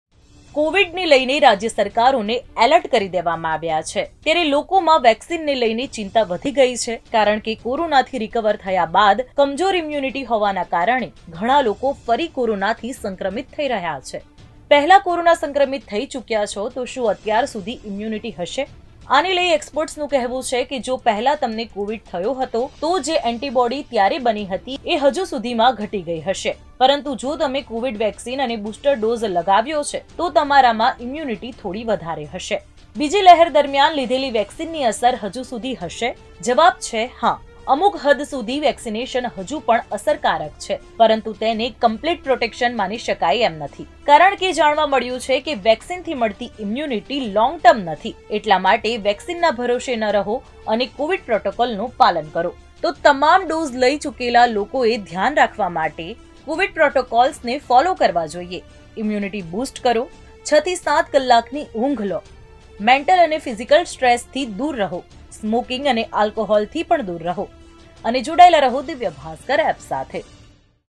કોરોના વેક્સિન કારગર છે કે નહીં? જે લોકોએ બૂસ્ટર ડોઝ નથી લીધો તેમને કેટલું જોખમ?
કોરોના વેક્સિન કારગર છે કે નહીં? જે લોકોએ બૂસ્ટર ડોઝ નથી લીધો તેમને કેટલું જોખમ? 3 એક્સપર્ટ પાસેથી જાણીએ A TO Z માહિતી